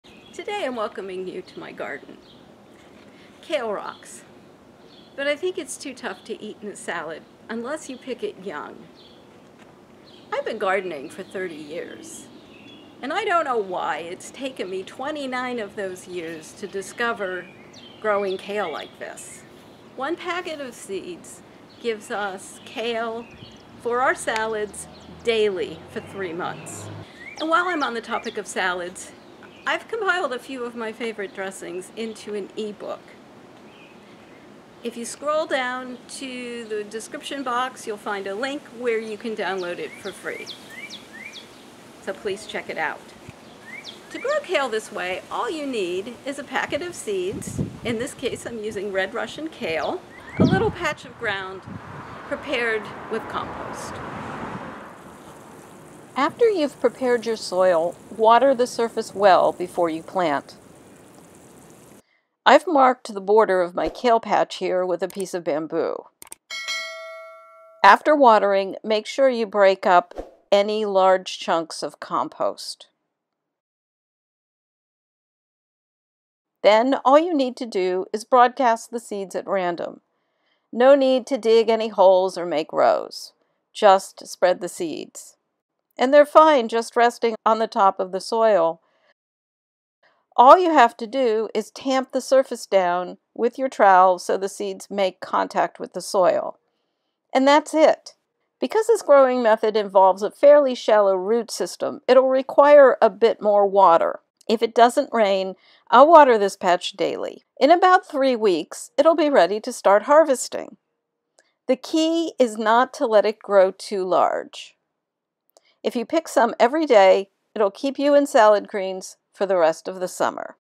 Grow an Endless Supply of Baby Kale for Your Salads All Summer!
Kale rocks! But not raw in a salad, it's just too tough. Baby kale to the rescue. It's easy to grow. One packet of seeds and some compost is all you need.